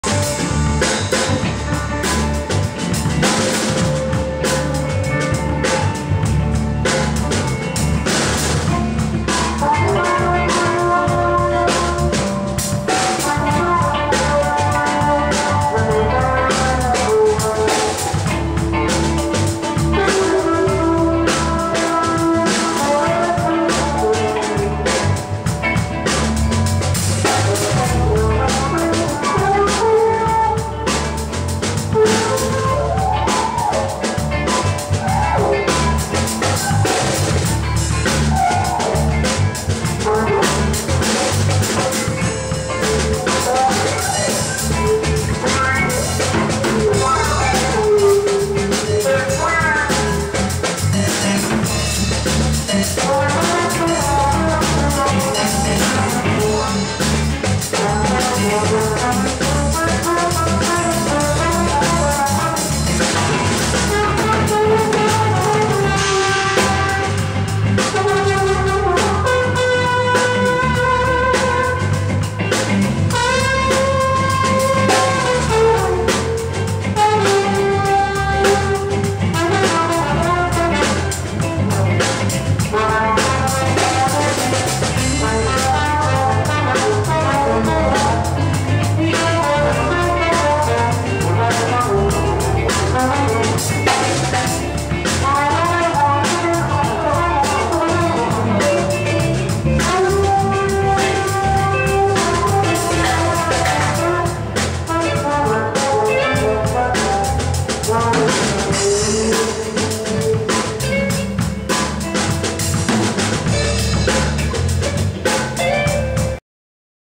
Dr Ed Byrne Trombone Jammin @ The New Little Shack Northampton Mass.3/12/10
Jammin @ The New Little Shack Northampton Mass. March 12th 2010 With Mark Herschler Guitar Vocals John Cruz Guitar Lincoln Hubley Drums Peter Kim Bass & Triggers & Dr. Ed Byrne Trombone.